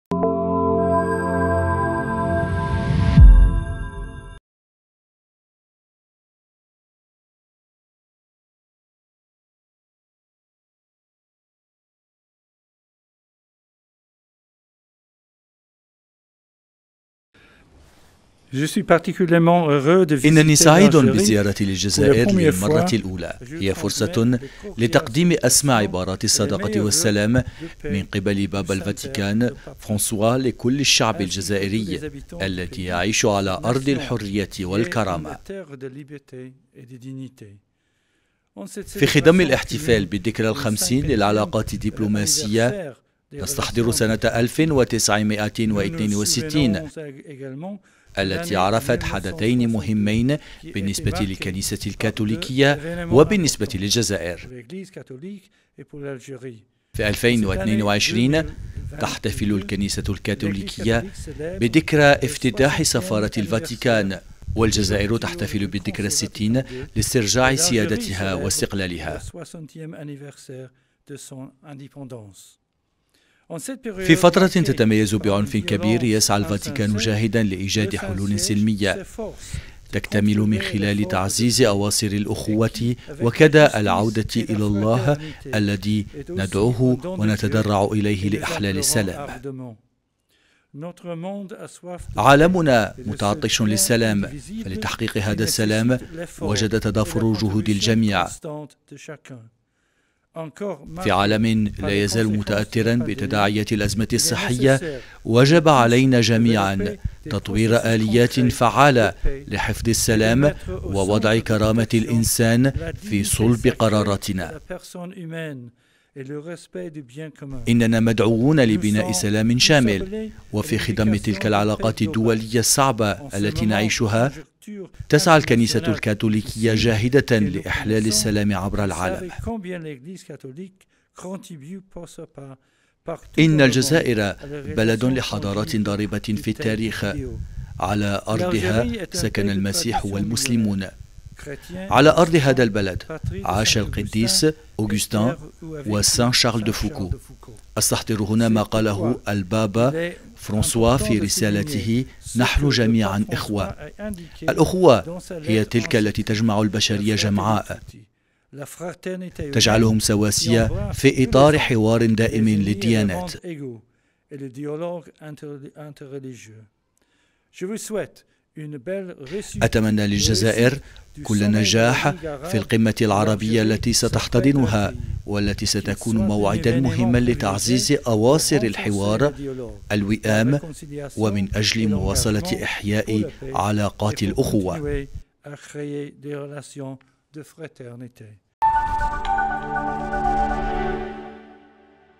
الرئيس تبون يستقبل رئيس الأساقفة بول ريتشارد غالاغر
الجزائر (وأج) -  استقبل رئيس الجمهورية, السيد عبد المجيد تبون, رئيس الأساقفة بول ريتشارد غالاغر, الكاتب المكلف بالعلاقات مع الدول والمنظمات الدولية بحاضرة الفاتيكان.(وأج)